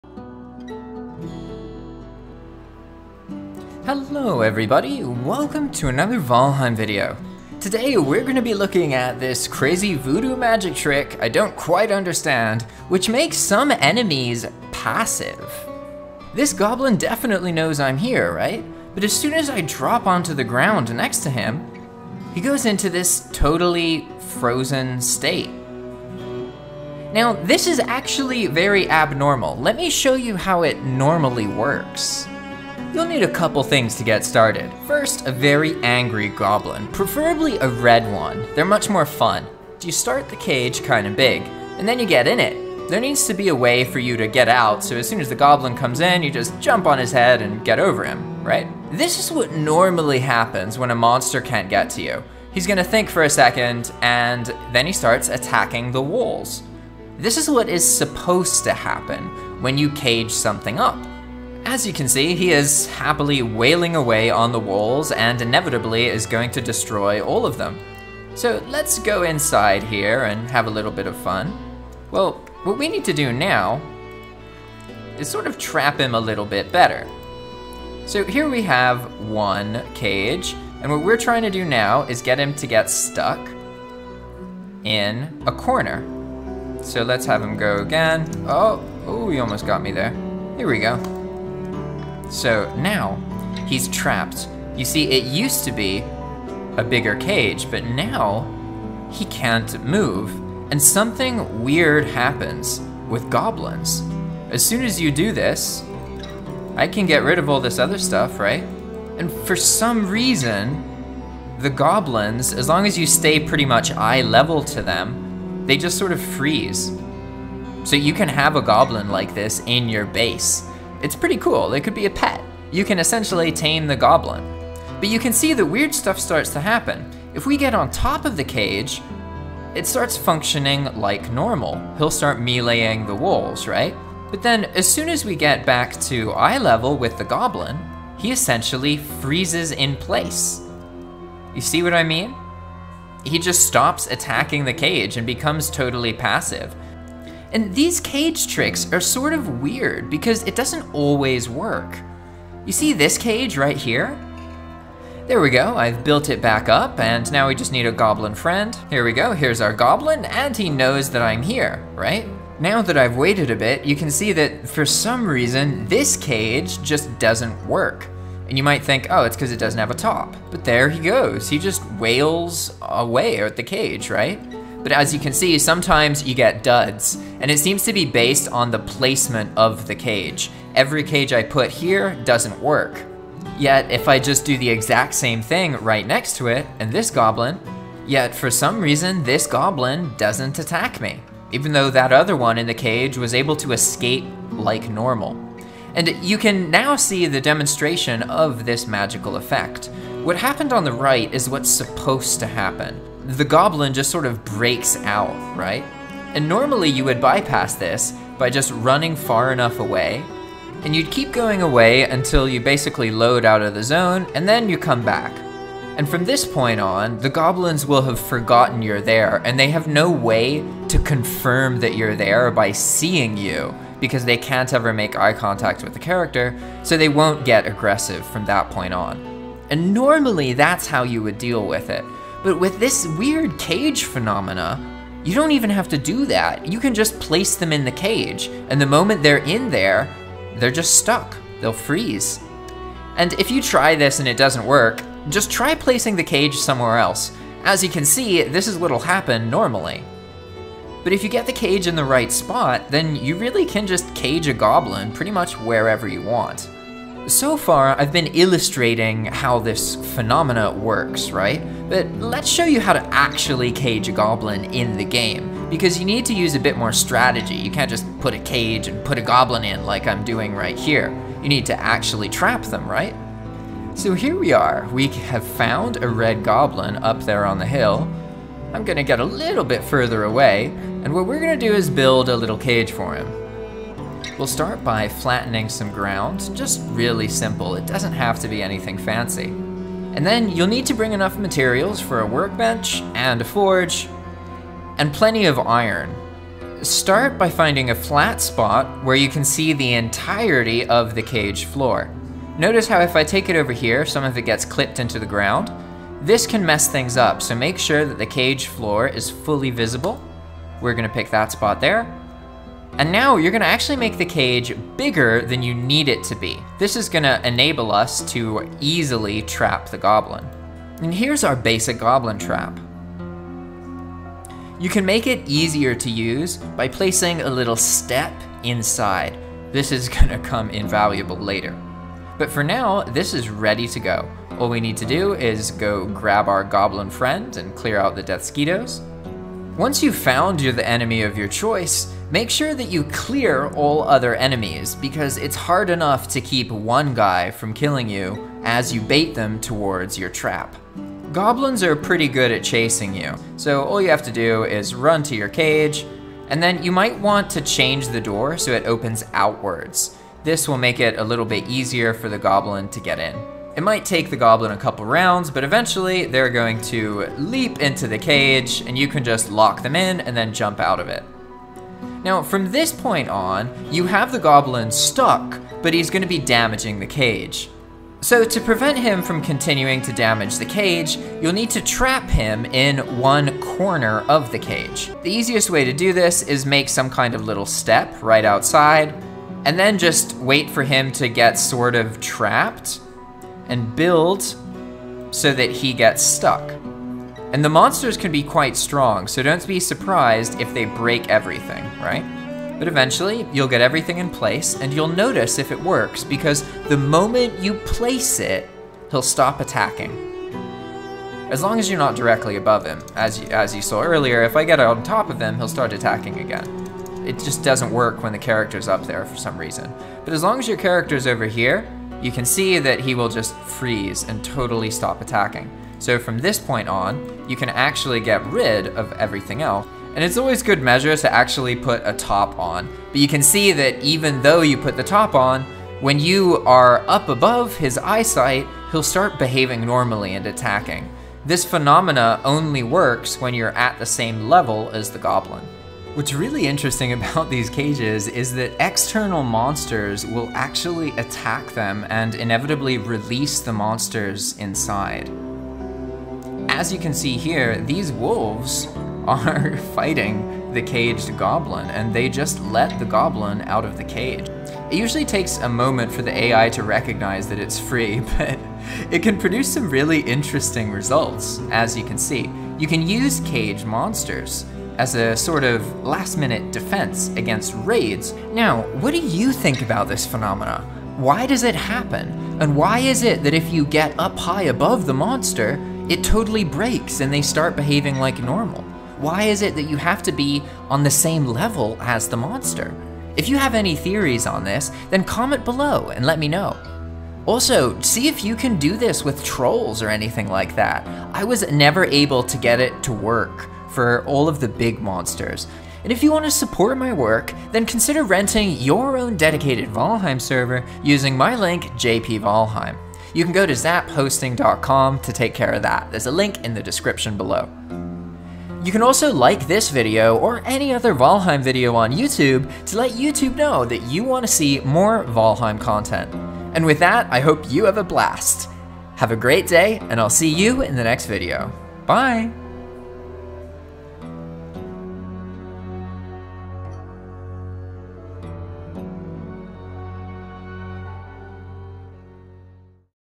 The cage trick that makes goblin passive
Chapters
00:00 Introduction
00:49 Normally monsters attack walls
01:31 The moment he becomes passive
02:29 Some spots do not work
04:17 How to trap a goblin
07:16 Wolves break cages